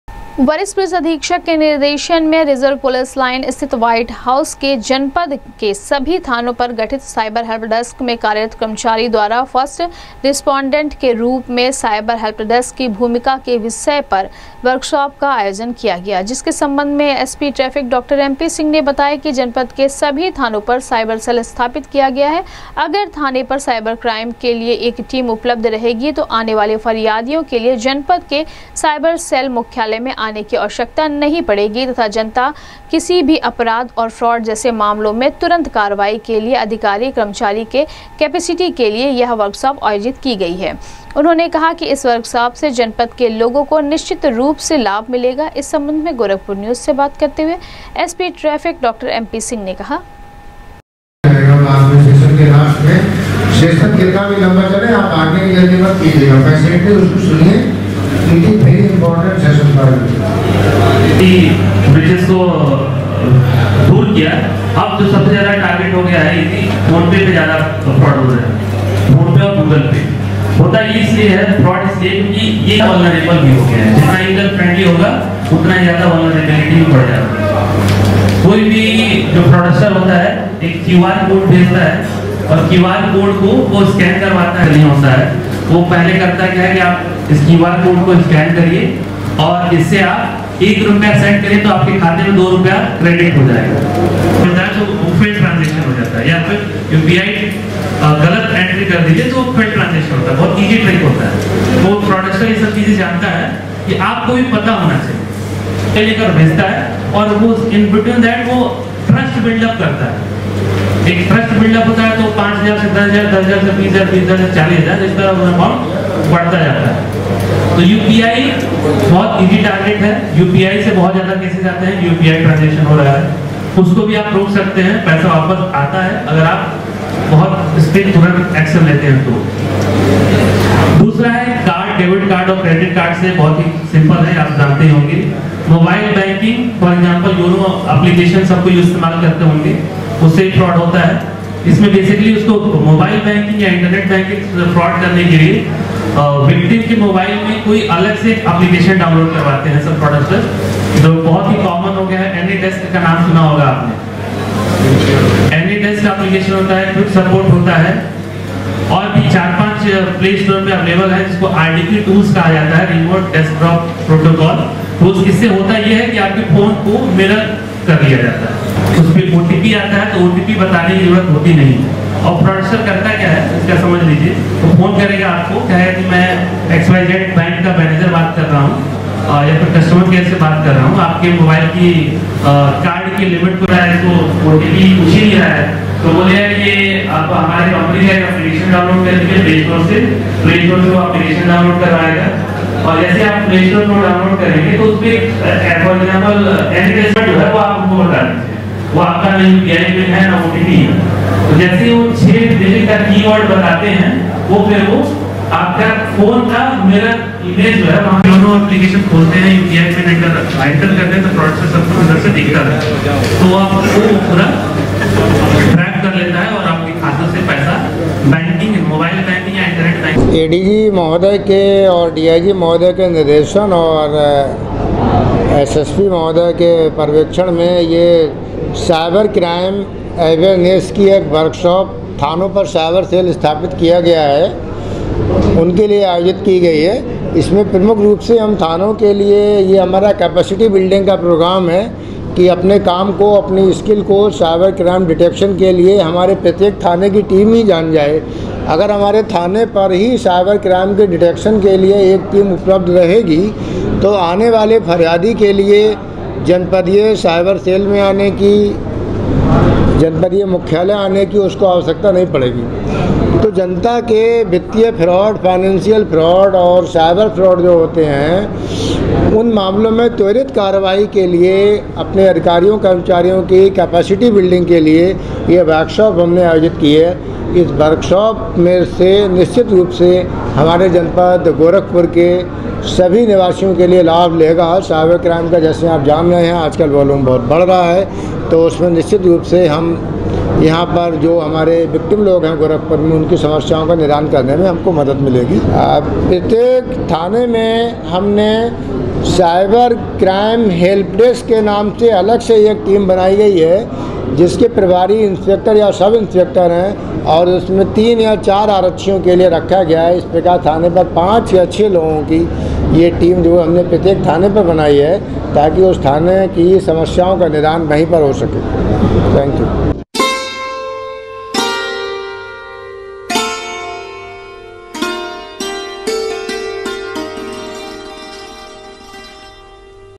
जनपद के सभी थानों पर गठित साइबर हेल्पडेस्क में कार्यरत कर्मचारी द्वारा फस्र्ट रिस्पाॅन्डेन्ट के रूप
जनपद के सभी थानों पर गठित साइबर हेल्पडेस्क में कार्यरत कर्मचारी द्वारा फस्र्ट रिस्पाॅन्डेन्ट के रूप में साइबर हेल्पडेस्क की भूमिका के विषय पर वर्कशॉप का किया गया आयोजन,कहा, सभी थाने पर साइबर क्राइम के लिए एक साइबर सेल की टीम उपलब्ध होने से फरियादियों को मुख्यालय आने की नहीं पड़ेगी आवश्यकता